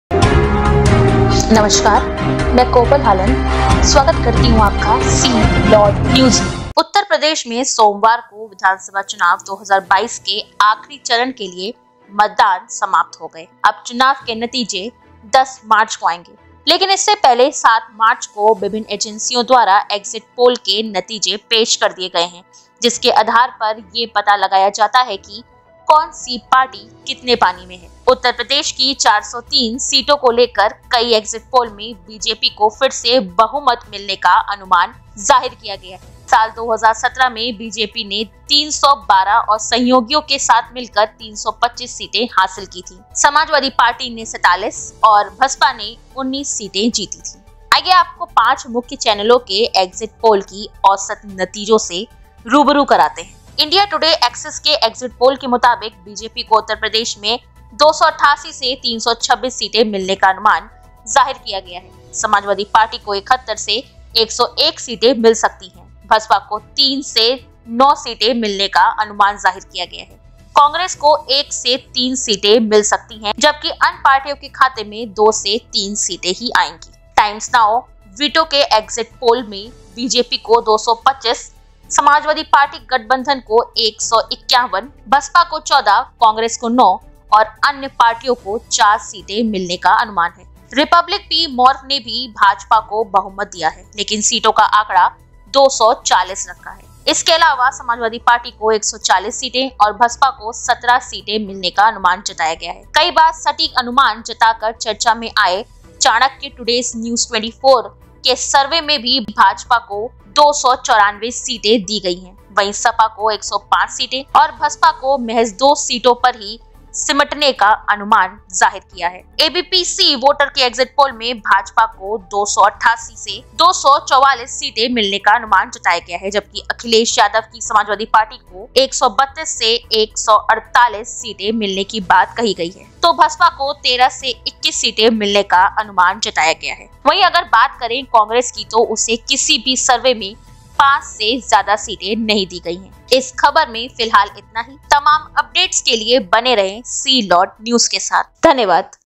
उत्तरप्रदेश विधानसभा चुनाव || विभिन्न Agencies द्वारा Exit Poll के नतीजे पेश किए गए || SeaLord News
उत्तर प्रदेश में सोमवार को विधानसभा चुनाव 2022 के आखिरी चरण के लिए मतदान समाप्त हो गया है। अब चुनाव के नतीजे 10 मार्च को आएंगे, लेकिन इससे पहले सात मार्च को विभिन्न एजेंसियो द्वारा एग्जिट पोल के नतीजे पेश कर दिए गए है जिसके आधार पर ये पता लगाया जाता है कि कौन सी पार्टी कितने पानी में है। उत्तर प्रदेश की 403 सीटों को लेकर कई एक्ज़िट पोल में बीजेपी को फिर से बहुमत मिलने का अनुमान जाहिर किया गया है. साल 2017 में बीजेपी ने 312 और सहयोगी के साथ मिलाकर 325 सीटें हासिल की थीं. समाजवादी पार्टी ने 47 और बसपा ने 19 सीटें जीती थीं.
आईये आपको पांच मुख्य चैनलों के एक्जिट पोल की औसत नतीजे से रूबरू करते है।
1. इंडिया टुडे एक्सिस के एक्ज़िट पोल के मुताबिक बीजेपी को उत्तर प्रदेश में 288  से 326 सीटें मिलने का अनुमान जाहिर किया गया है.
    समाजवादी पार्टी को 71 से 101 सीटें मिल सकती हैं. बसपा को    तीन से नौ सीटें मिलने का अनुमान जाहिर किया गया है.
कांग्रेस को एक से तीन सीटें मिल सकती हैं. जबकि अन्य पार्टियों को दो से तीन सीटें मिल सकती हैं.
2. टाइम्स नाऊ-वेटो के एक्ज़िट पोल में बीजेपी को 225, समाजवादी पार्टी गठबंधन को 151, बसपा को 14, कांग्रेस को नौ और अन्य पार्टियों को चार सीटें मिलने का अनुमान है.
3. रिपब्लिक पी. मार्क ने भी भाजपा को बहुमत दिया है, लेकिन सीटों का आंकड़ा 240 रखा है। इसके अलावा समाजवादी पार्टी को 140 सीटें और बसपा को 17 सीटें मिलने का अनुमान जताया गया है।
4. कई बार सटीक अनुमान जताकर चर्चा में आए चाणक्य टुडेज-न्यूज 24 के सर्वे में भी भाजपा को 294- सीटें दी गई हैं। वहीं सपा को 105 सीटें और बसपा के महज 2 सीटों पर ही सिमटने का अनुमान जाहिर किया है।
5. एबीपी-सी वोटर के एग्जिट पोल में भाजपा को 288- से 244- सीटें मिलने का अनुमान जताया गया है, जबकि अखिलेश की समाजवादी पार्टी को 132 से 148- सीटें मिलने की बात कही गई है। तो बसपा को 13 से 21 सीटें मिलने का अनुमान जताया है।
वहीं अगर बात करे कांग्रेस की तो उसे किसी भी सर्वे में 5 से ज्यादा सीटें नहीं दी गई हैं।